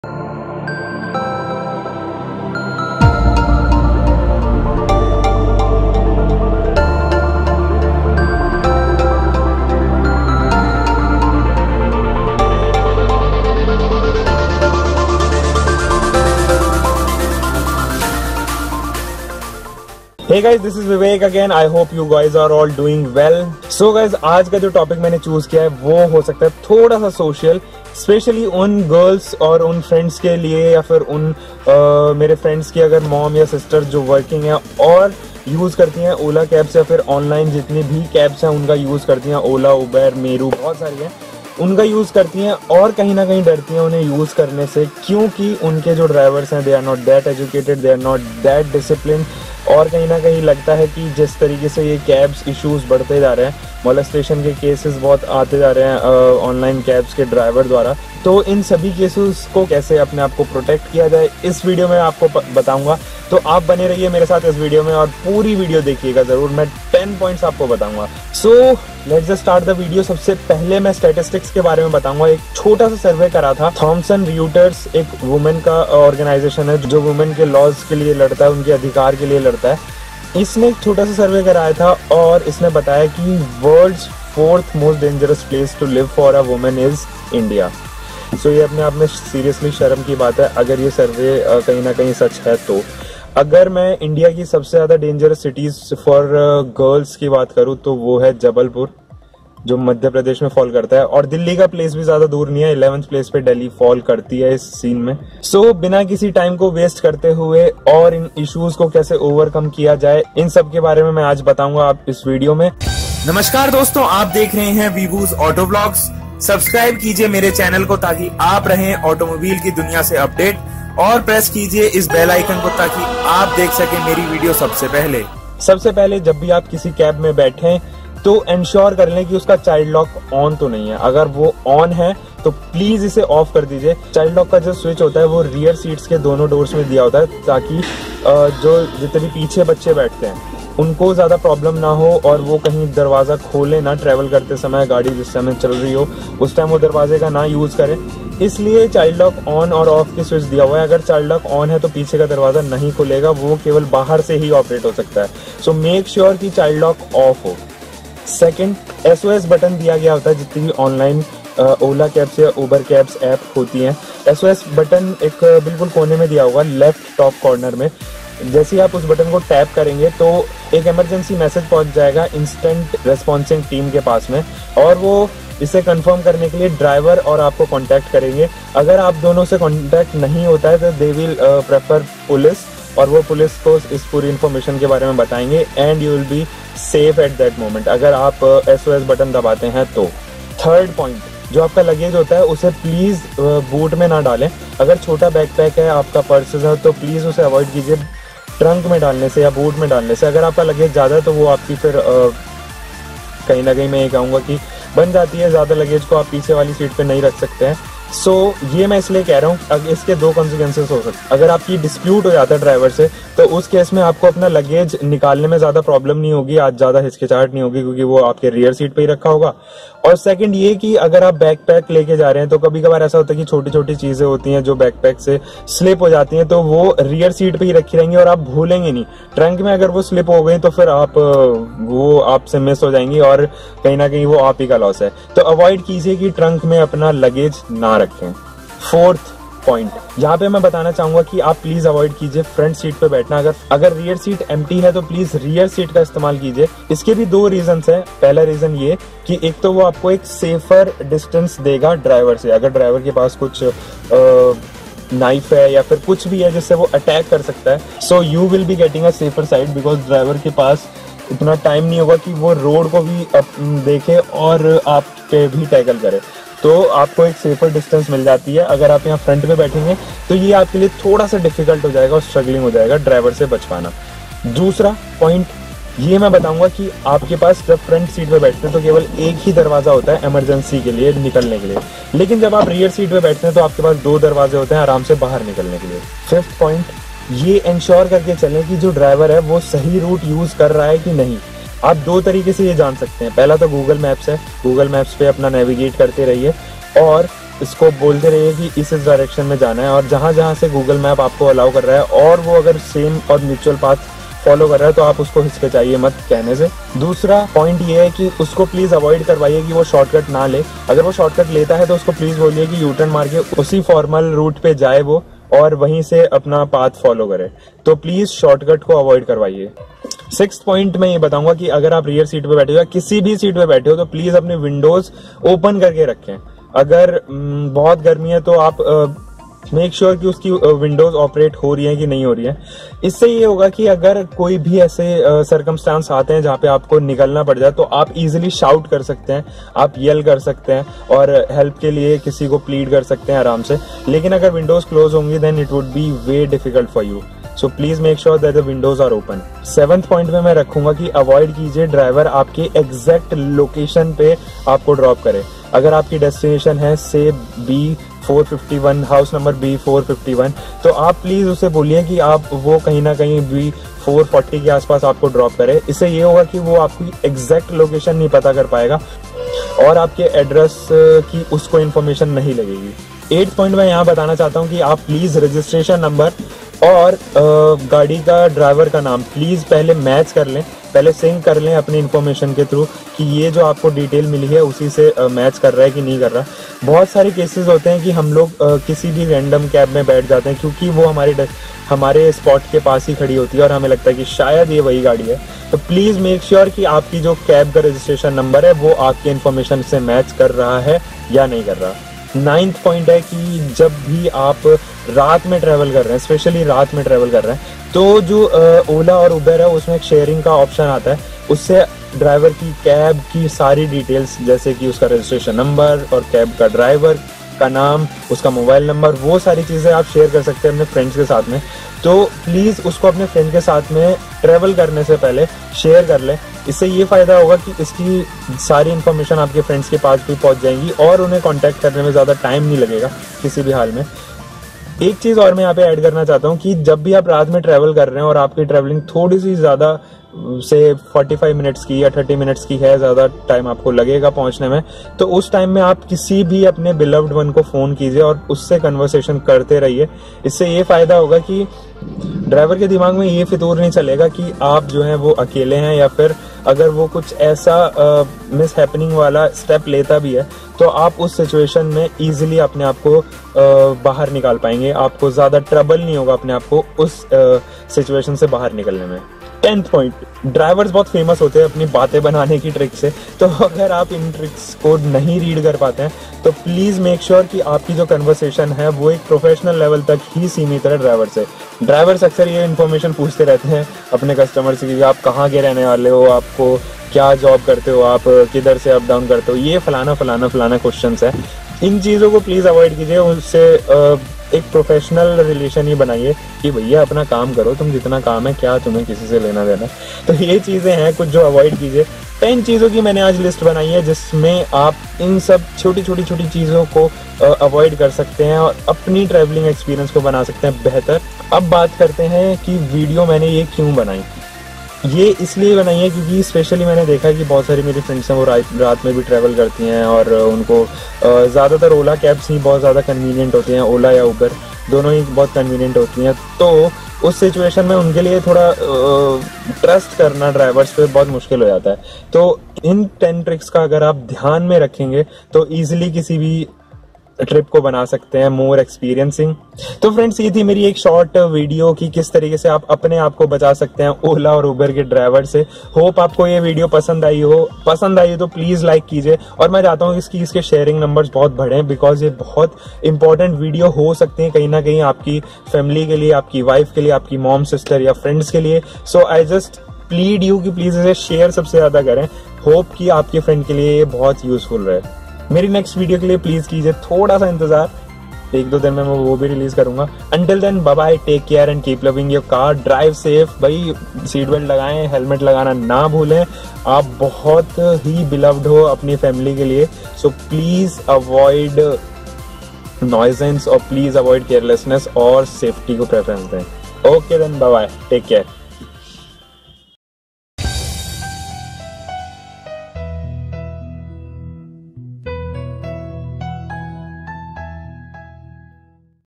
10 TIPS to stay safe in OLA/UBER/any other CAB | must watch for everyone
This video demonstrates the ways how to put yourself safe when using an OLA or UBER . Watch this video, may be this video could help you put your fear away now.

-vivu